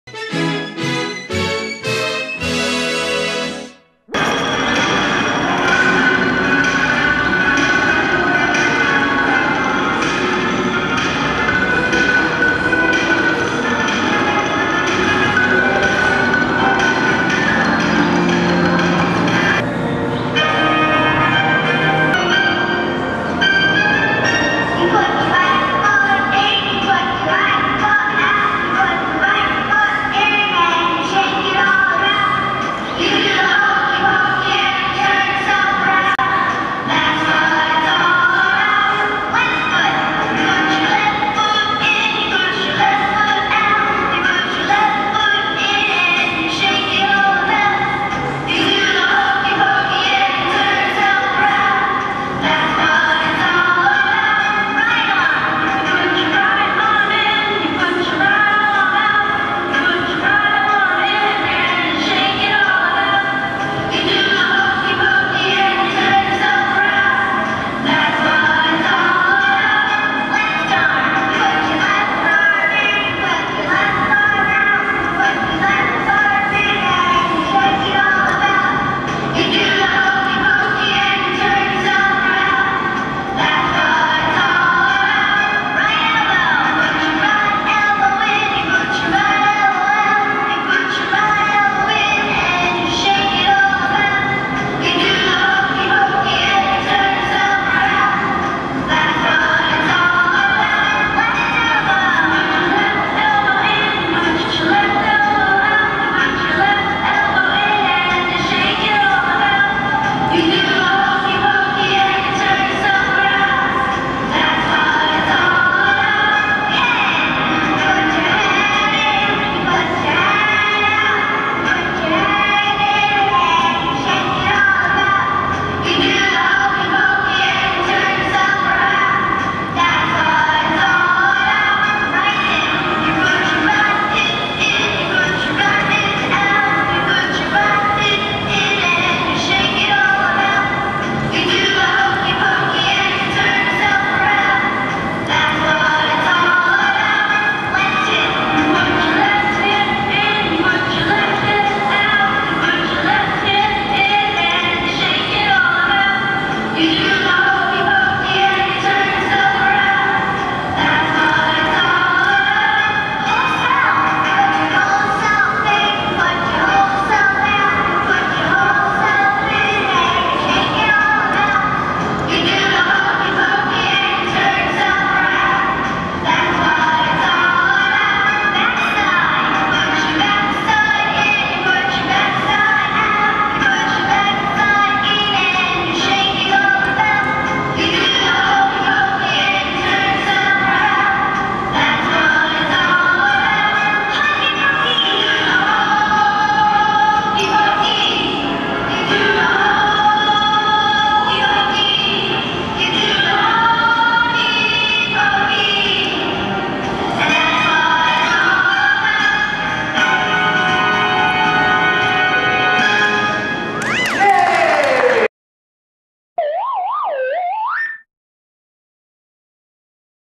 Maya Butterflya and Mr Bones Skeleton Marionette act
From the "Make-Believe Magic Dragon Family Wonder Show (of the Universe)" as seen at malls around the Bay Area, Maya Butterflya presents something a little fanciful for Halloween.